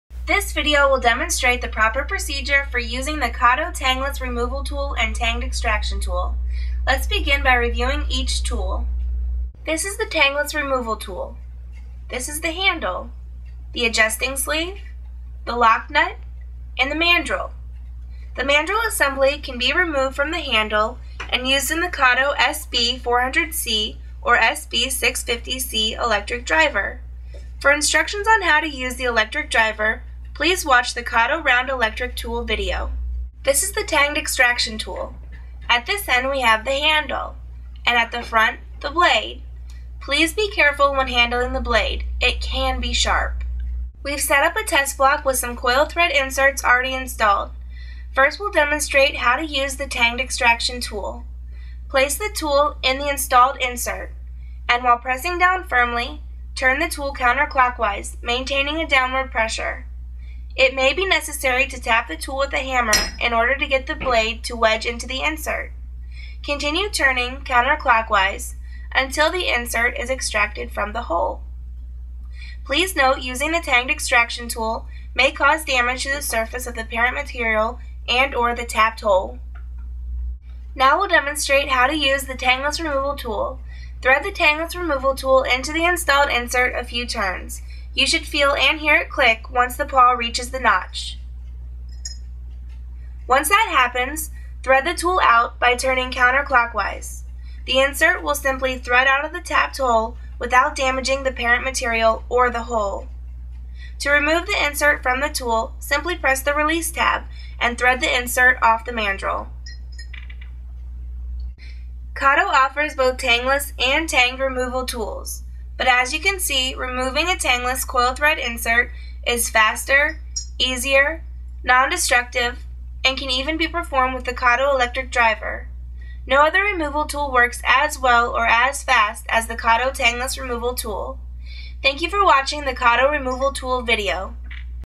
KATO Extraction & Removal (KRT) - For Tangless & Helicoil inserts
This video will demonstrate the proper procedure for using the KATO Tangless Removal tool and Tanged Extraction tool.